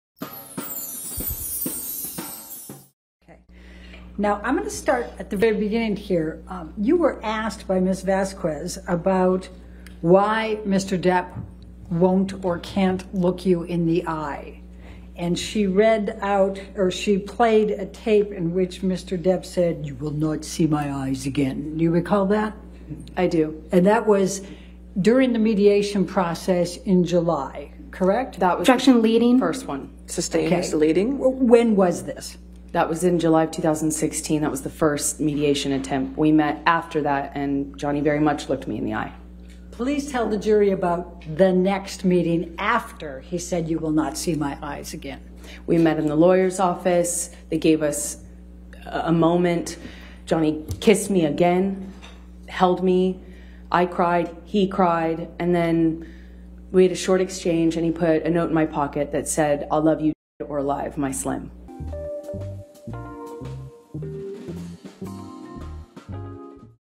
Abogada de Amber se mete con Johnny Depp imitandolo | ENTREVISTA SUBTITULADA
¡Me gusta, comenta y comparte!